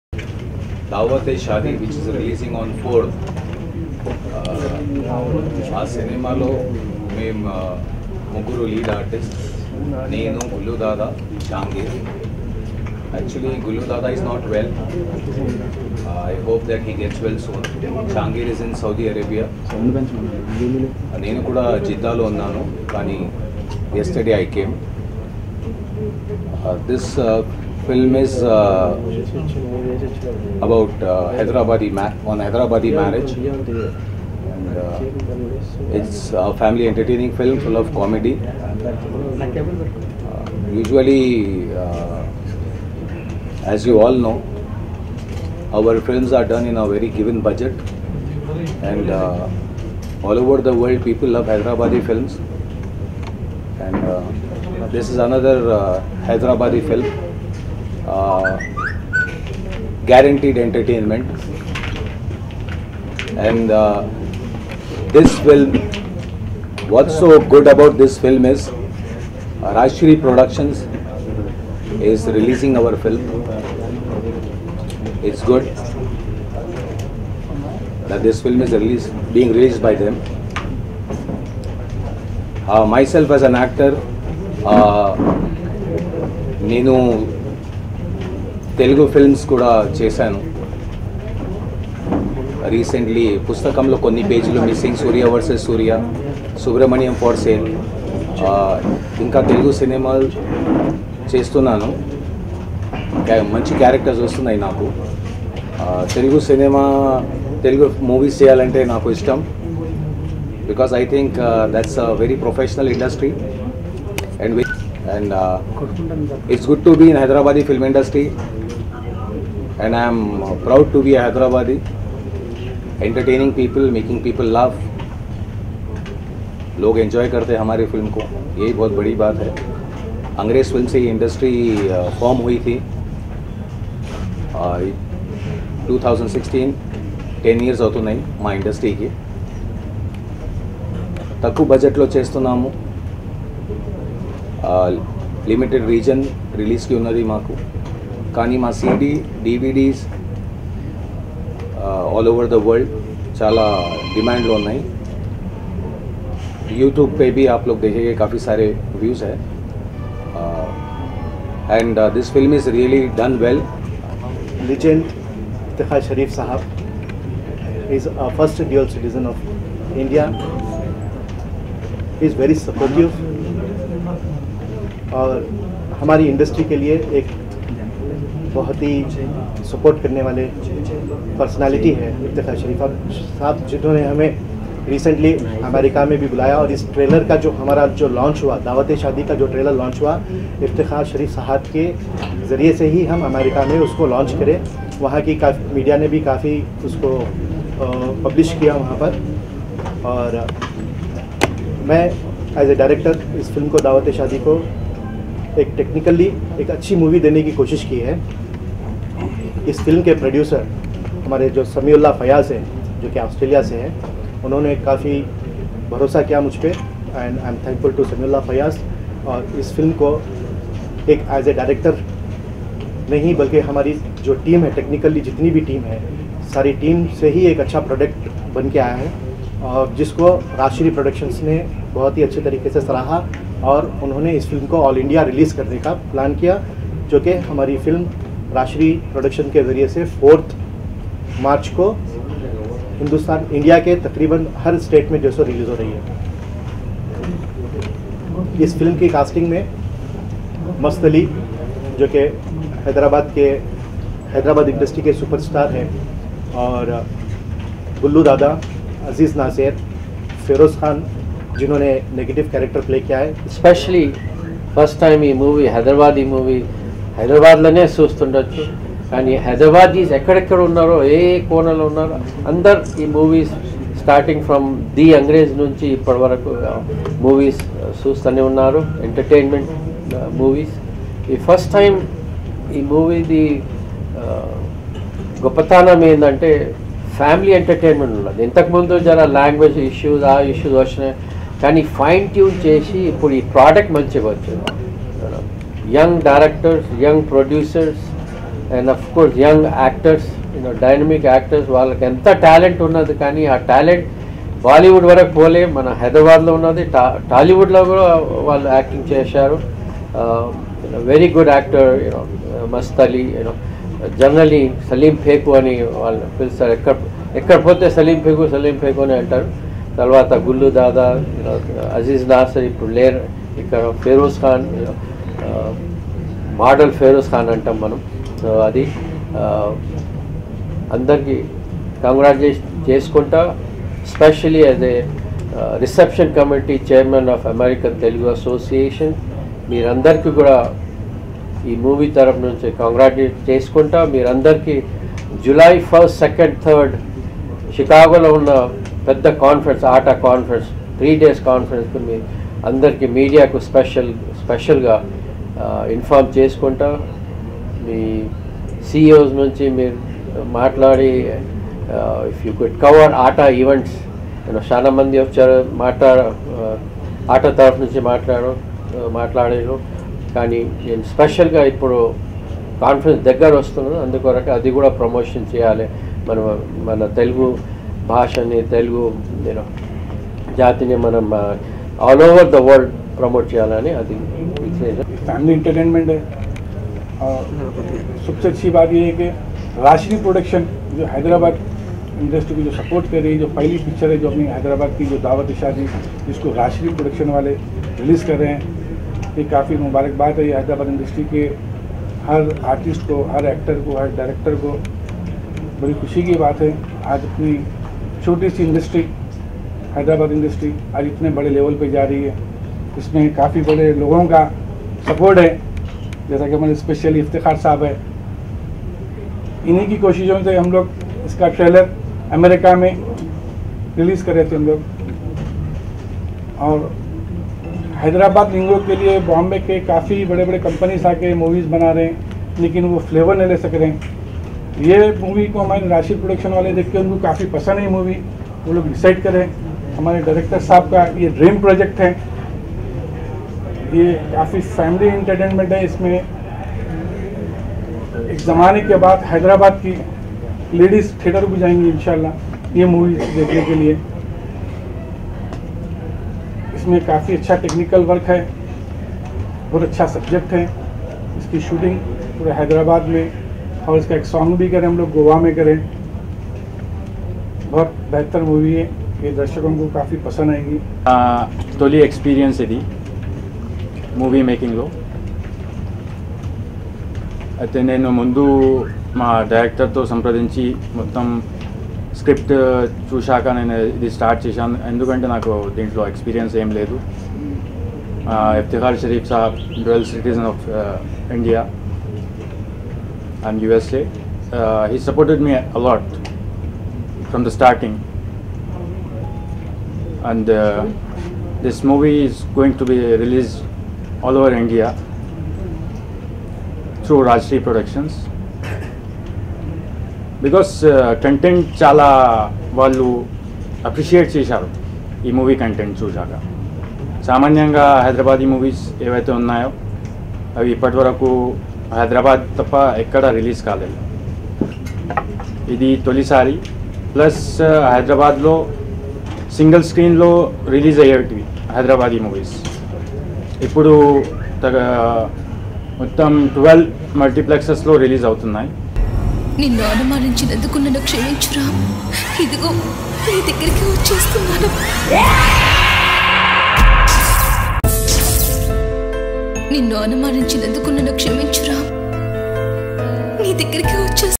Dawat E Shaadi Movie Press Meet | Adnan Sajid Khan, Mast - Ali, Syed Hussain
Dawat E Shaadi Movie Press Meet

Starring : Adnan Sajid Khan, Mast - Ali, Aziz Naser, Feroz Khan, Madhavi, Manisha, Kavya
Written and Directed by  Syed Hussain
Produced by  Sami Ullah Fayaz